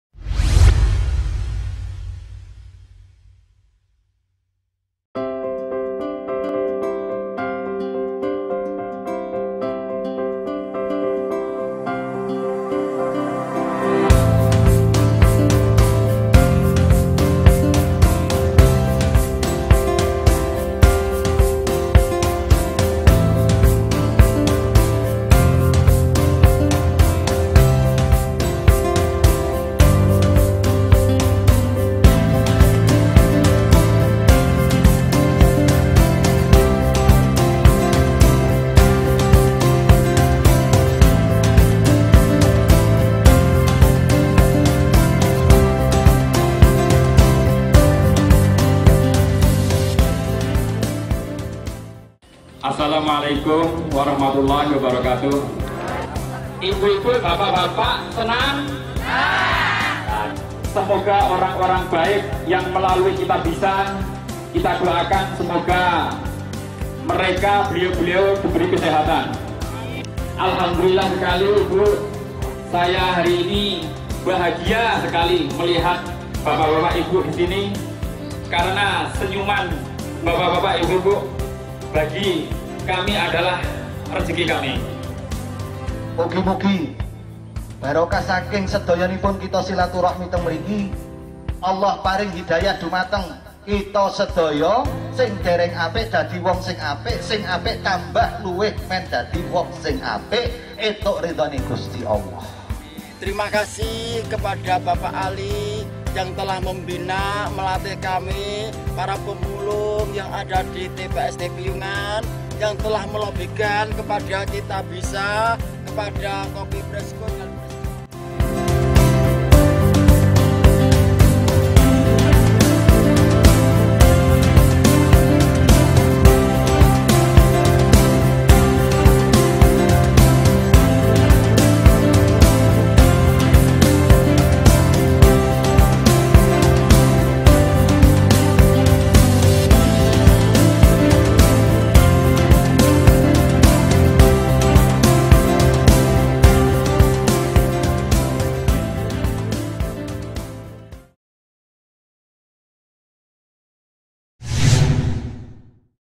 IKHLAS TANPA BATAS
Aku,
Bukanlah siapa2 tanpa mereka.
Bukan apa2 tanpa mereka.
Aku hanyalah hamba yang di taqdirkan Tuhan untuk mengantarkan amanahNya,
Perantara orang2 baik yang menyisihkan rizkinya dan menyalurkannya padaku,
Untuk mereka yang membutuhkannya.
Senyum mereka adalah bahagiaku,,
Bahagia mereka adalah segala rasa syukurku,,
Terimakasih Tuhan telah mengirimkan banyak orang baik disekelilingku,
Untuk membantuku membahagiakan mereka saudara2ku,
Hari ini,
Bakti sosial bersama 500 pemulung,
Melihat senyum rekah mereka, hati ini sangat bahagia.
Semoga langkahku selalu diberkahi olehNya, perantara do'a2 mereka,
Do'a yang mustajabah dari saudara2ku dimanapun tempatnya.

Trimakasih untuk orang2 baik.
Semoga kita sama2 ditempatkan di surgaNya kelak.